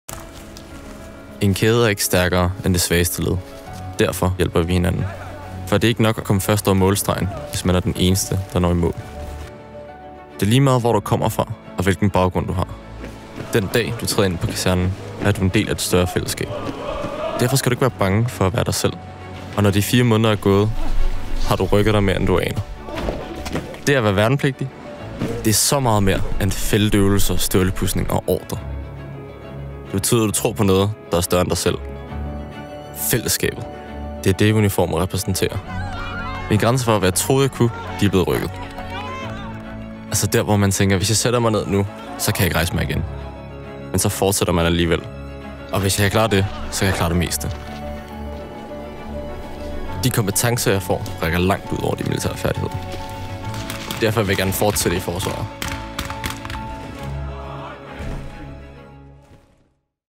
Hvordan er det at være værnepligtig?
"Fællesskabet. Det er det, uniformen repræsenterer". Se videoen og læs mere om værnepligten på